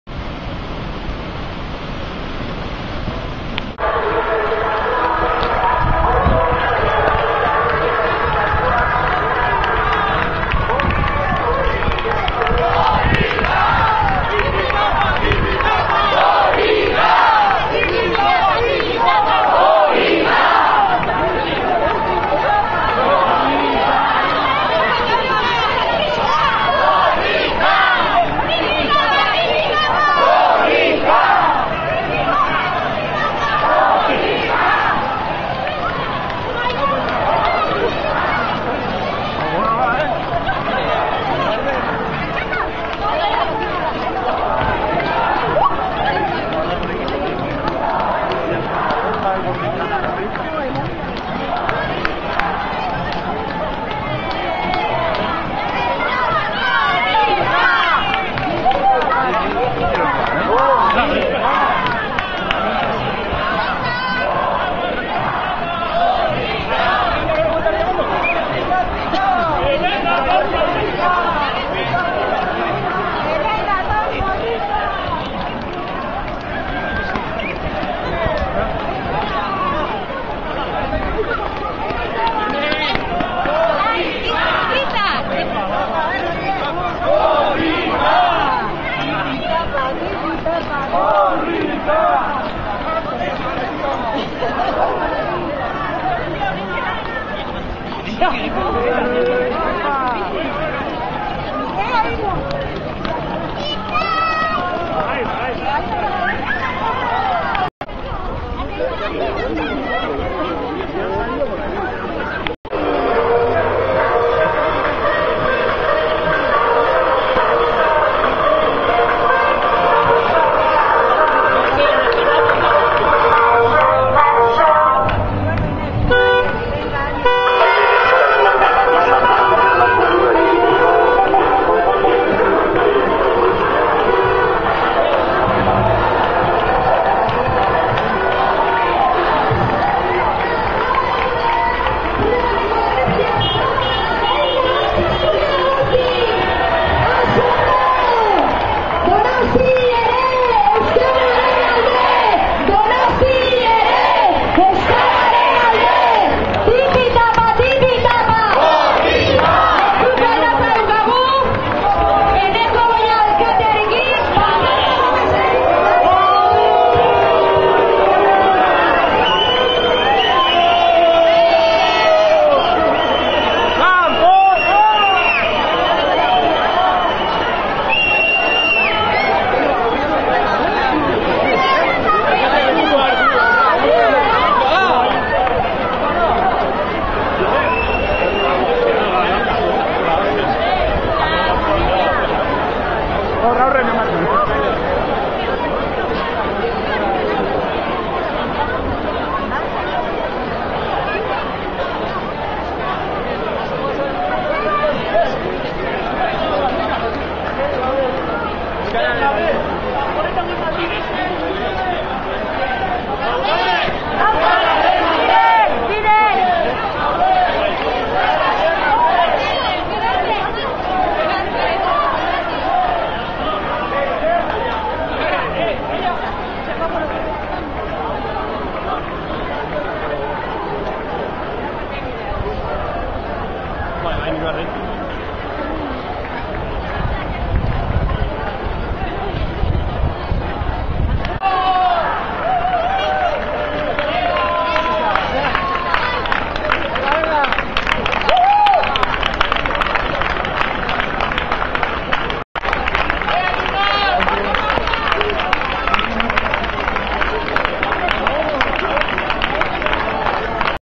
Korrika pirata Donostia 2019-4-12
Korrika Donostian kaia piraten lekukoa eta Bulebarra.